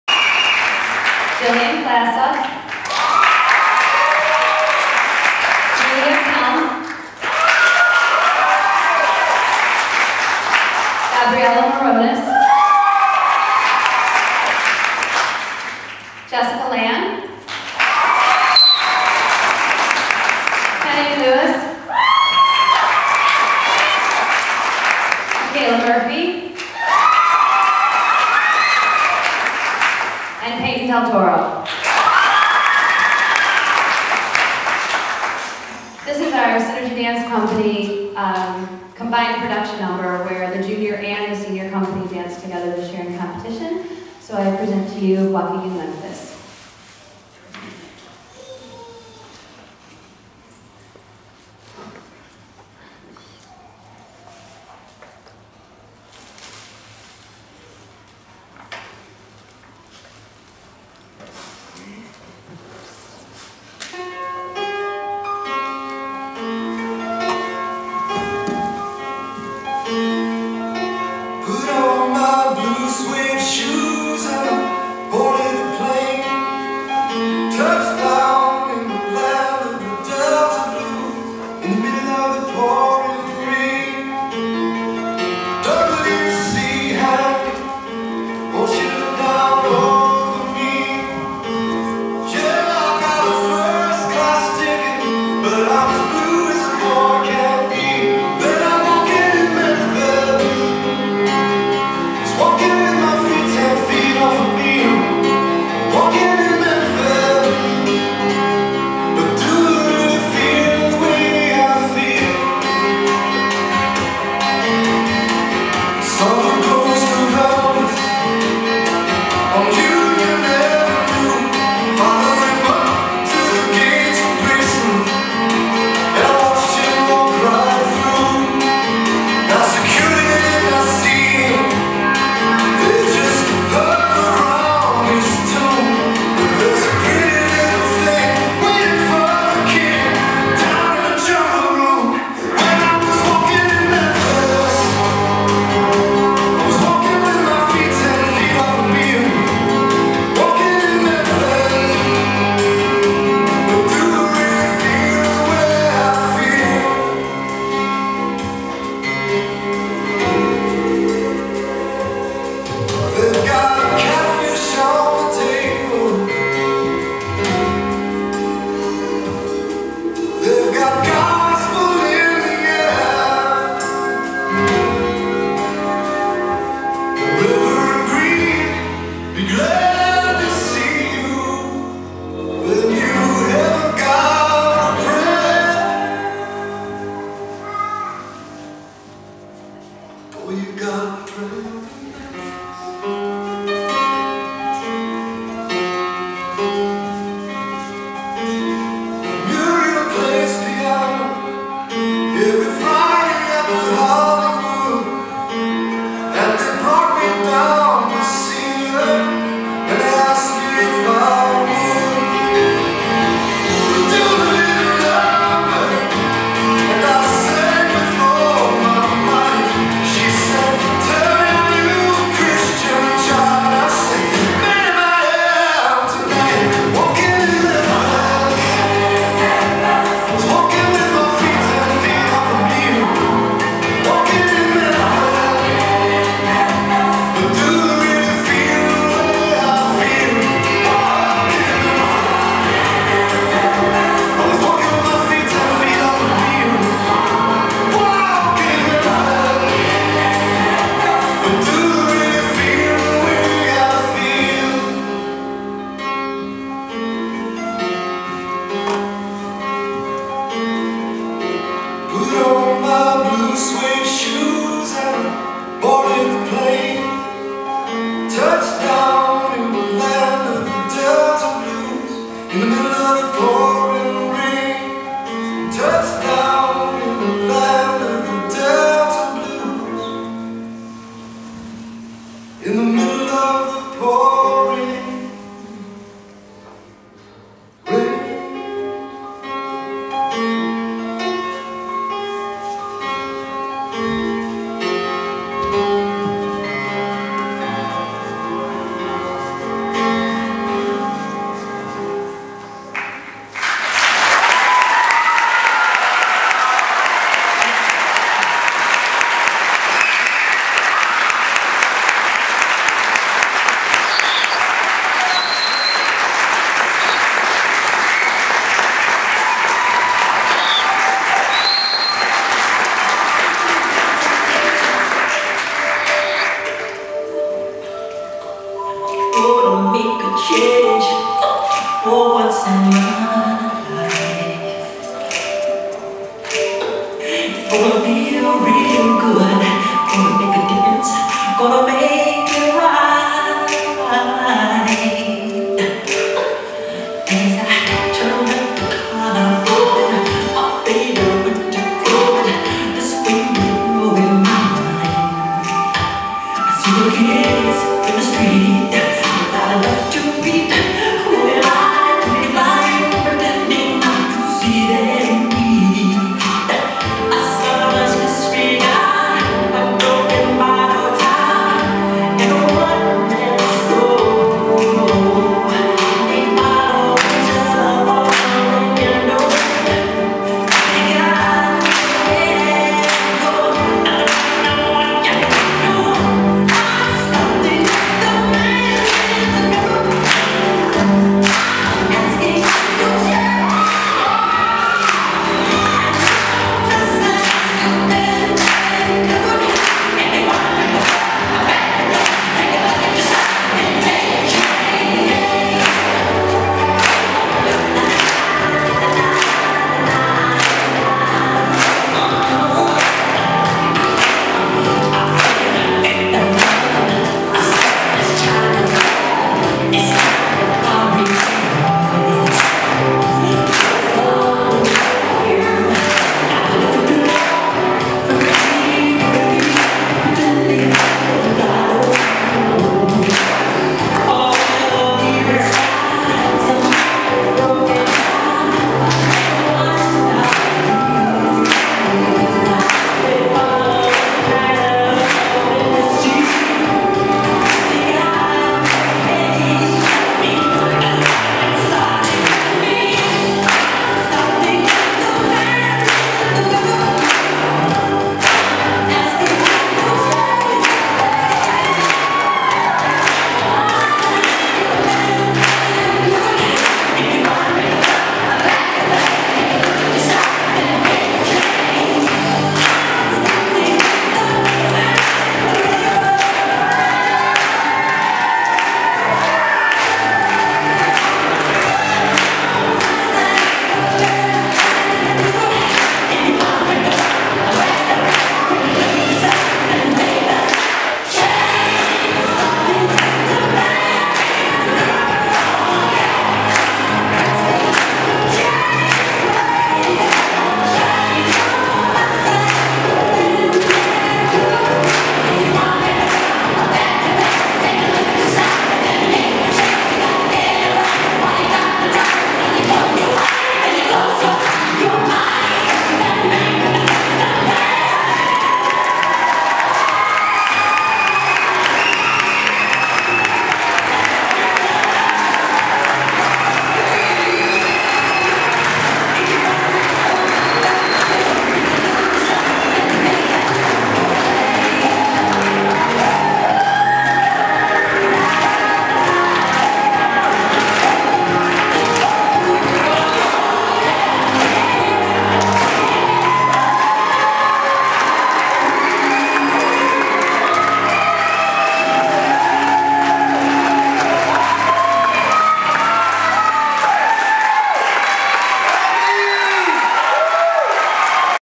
Jessica in SDA Competition Dance+Finale June 2010.MOD
Jessica and Riley were the youngest ones in the competitional team. Jessica went to Australia and China for a month from the end of Oct. to Nov.. She was in the front row at the beginning, but missed 4-week rehearsals when she's gone. Usually this would cause to be kicked out from the number, but Jessica was exceptional. I bet Lynn really wanted to keep her in this number. As Lynn said at the recital, Jessica can dance many dances as she wants, she is a hard worker and great dancer.